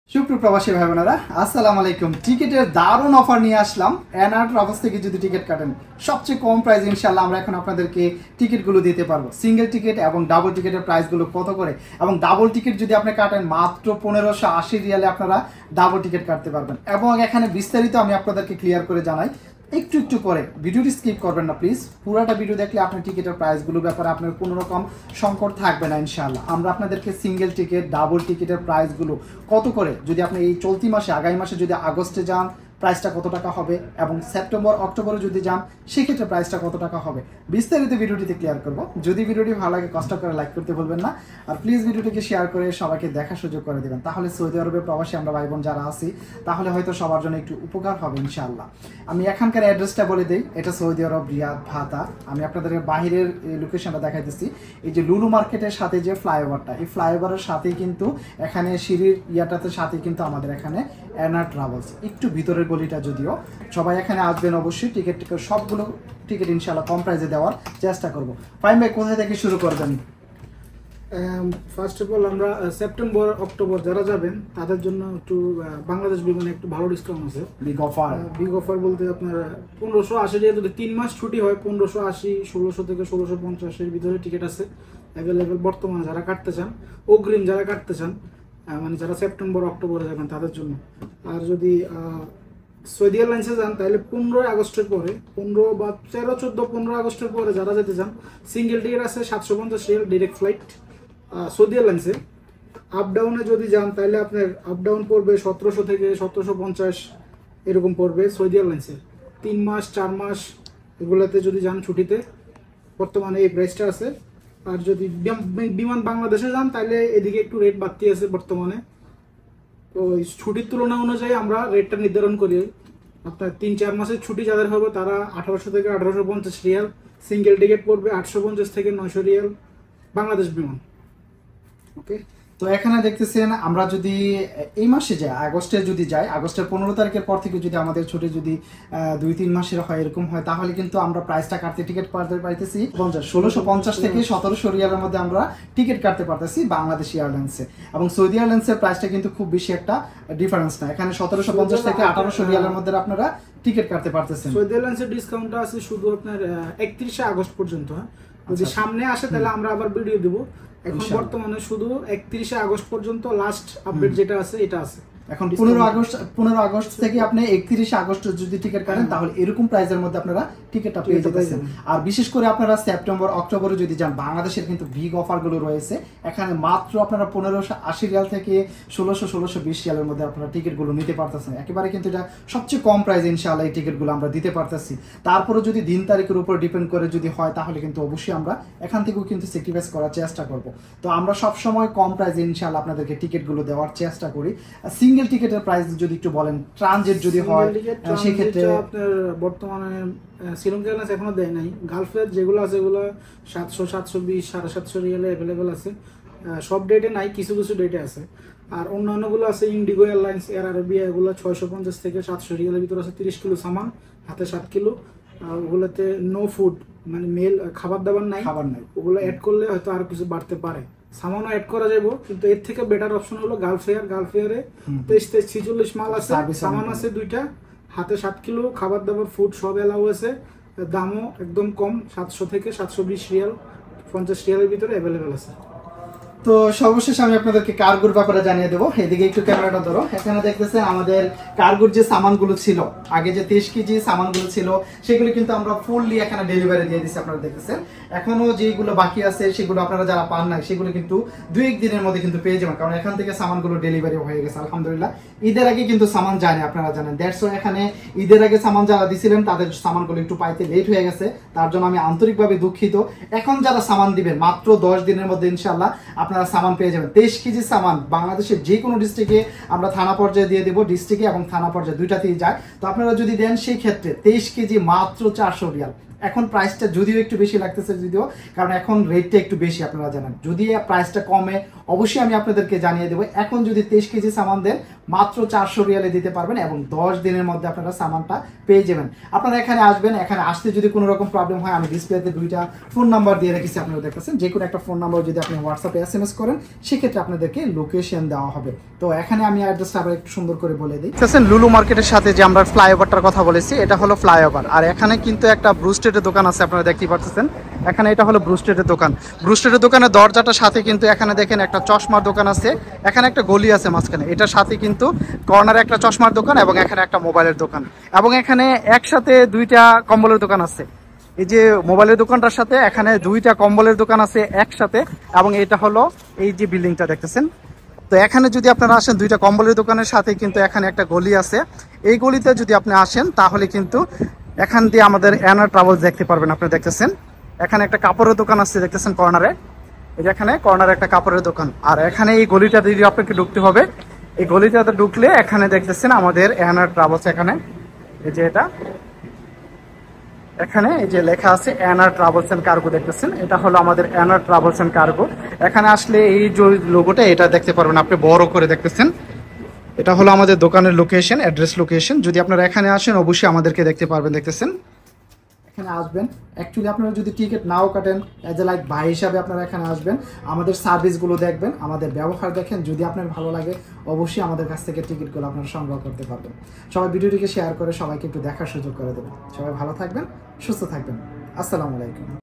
বিমান টিকেটের দারুণ অফার, কমেছে বিমানের টিকেটের দাম ৩০/০৭/২৪,
রিয়াদ বাথা থেকে বিমান টিকেটের দাম কত বিস্তারিত ভিডিও তে বলার চেষ্টা করলাম ,